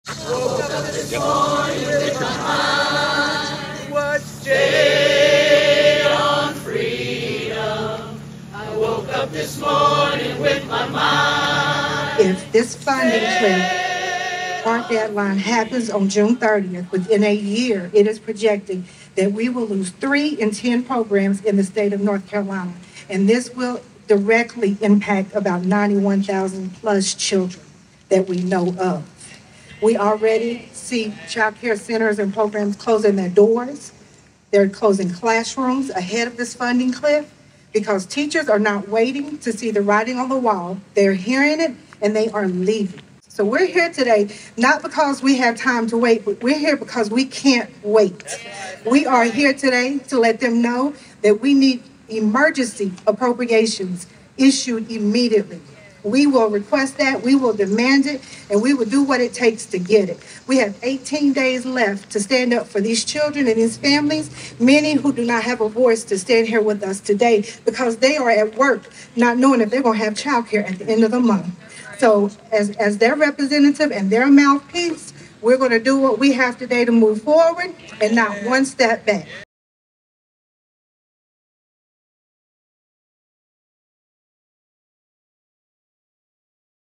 Child care funding ‘cliff’ is near. Advocates call on NC leaders to take action.
Federal money that helped stabilize the child care industry in North Carolina is set to run out at the end of June. Advocates are calling on state lawmakers to take action.
Video by Luciana Perez Uribe Guinassi / The News & Observer